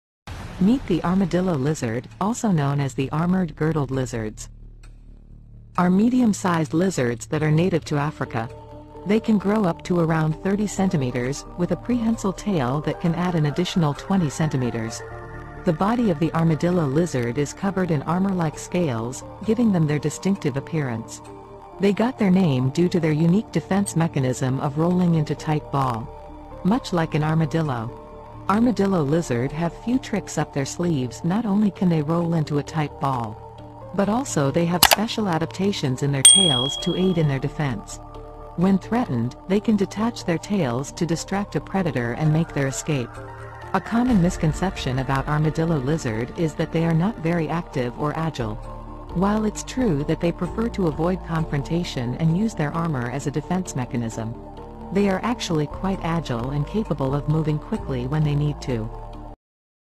Amazing Armadillo Lizard Facts and Behavior
Discover the fascinating world of the Armadillo Lizard, also known as the African Armadillo or the Armadillo Girdled Lizard. This small, scaly reptile is native to Southern Africa and is known for its unique appearance and behavior. In this video, we will explore the incredible features and adaptations that make the Armadillo Lizard one of the most interesting lizards in the world.